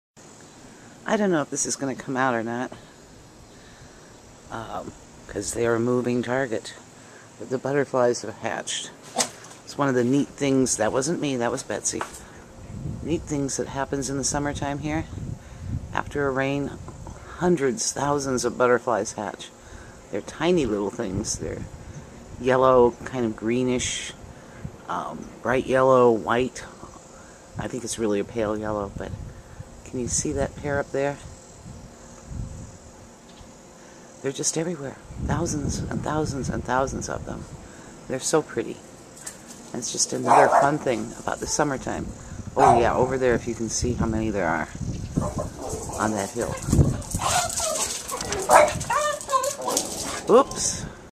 Butterflies
After a rain the butterflies hatch and are just so beautiful. This year, with so much rain, it seems like there are trillions of them! Wherever you look there are butterflies. I tried to capture it with my Flip camera, can you see them all? There must be two hundred or more that flew around when I was filming.